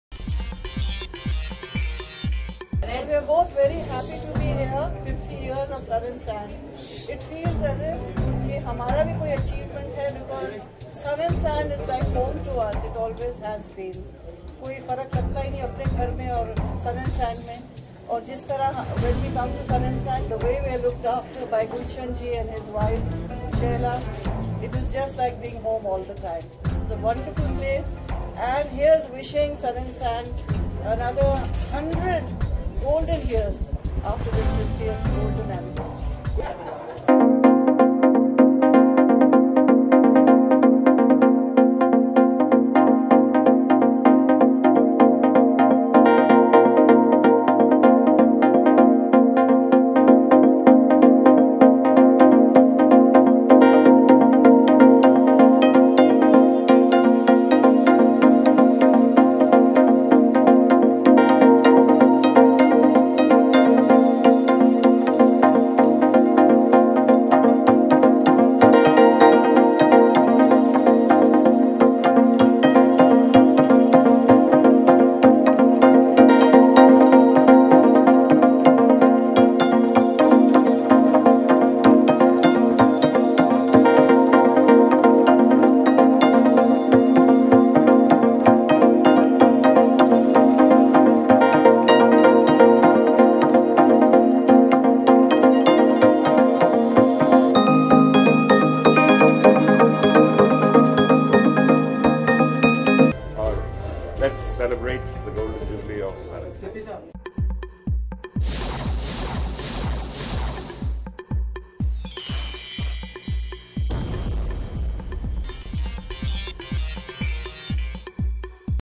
Bollywood Celebs At 50 years Celebration Sun 'N' Sand Bash
Celebrities Like Jitendra, Saira Banu, Dilip Kumar and many more spotted at the 50 years celebration of Sun 'N' Sand bash